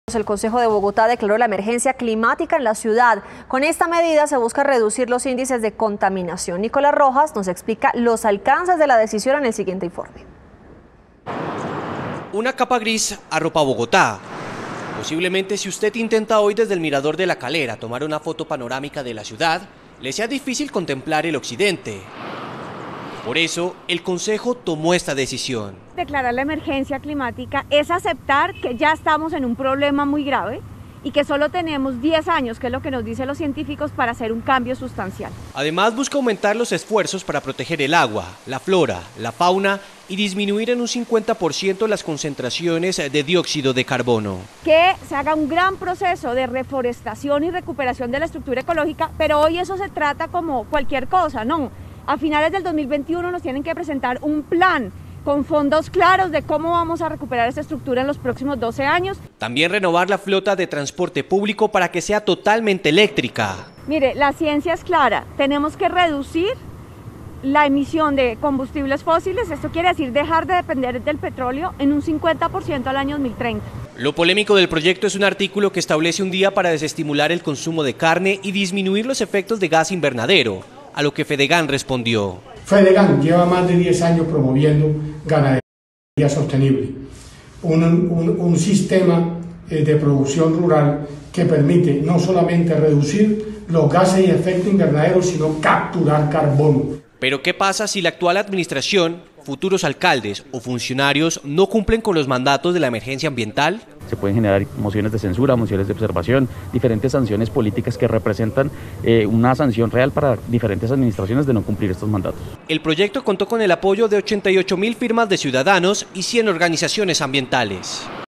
Declaran emergencia climática en Bogotá y se establece el "día sin carne"
El Concejo de Bogotá declaró la emergencia climática en la ciudad, medida con la que se pretende reducir los índices de contaminación a partir de acciones que protejan el agua, la flora y la fauna.

Otras de las acciones es renovar la flota de transporte público para que sea eléctrica.

Lo polémico del proyecto es un artículo que establece un día para desestimular el consumo de carne y disminuir los efectos de gases invernadero, a lo que Fedegán respondió.